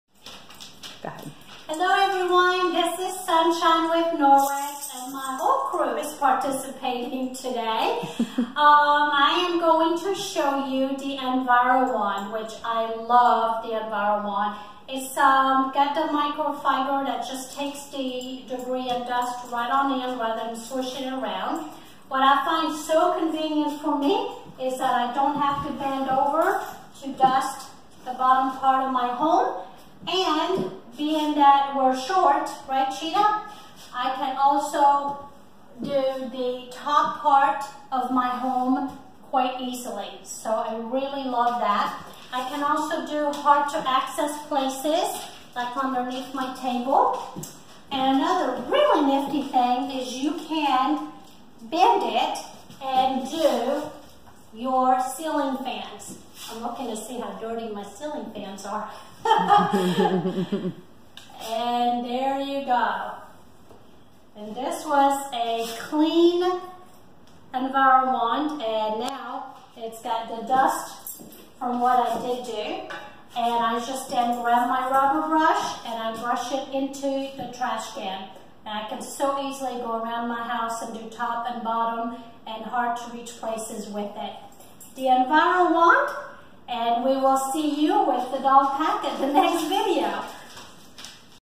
Norwex Envirowand: Reaching High and Low Places!
I love this EnviroWand because I can reach high and low places! Don't have to bend over to dust the baseboards and don't have to get the step ladder to reach high places including the ceiling fans.
And of course the Norwex microfibers pick up dust without swishing them around so it's effective, and all without additional products that draw dust back in.

Drop me a comment and let me know what you think?!